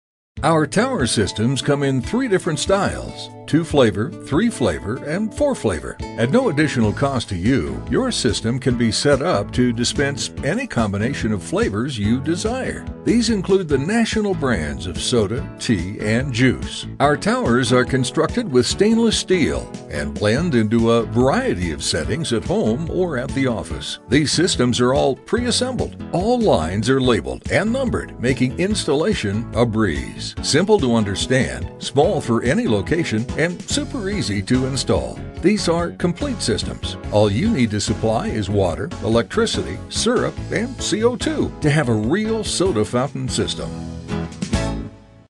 Home Soda Fountain: Tower
Counter top tower soda fountain system. This complete system is designed for home or office. The stainless dispenser is compact in size and blends in a variety of settings without eating up valuable counter space.

We will set up the system to accommodate your choice of post mix bag-in-box flavors at no additional cost.

Very simple hook-up. The lines have been pre-assembled & labeled. ALL of the fittings have been professionally clamped for easy installation.

Also included: Three (3)  syrup pumps and mounting bracket with BIB hosing and connects,  primary & secondary CO2 regulators along with a 5 lb. tank, high pressure hosing, carbonator, & a remote cooler. Smallest chiller on the market fits under any cabinet. Keeps product a correct serving temperature to prevent carbonation loss.

We offer FREE Technical Support and our Installation Guide (PDF) download—including links to the Start-up Procedures video for your system—with this purchase.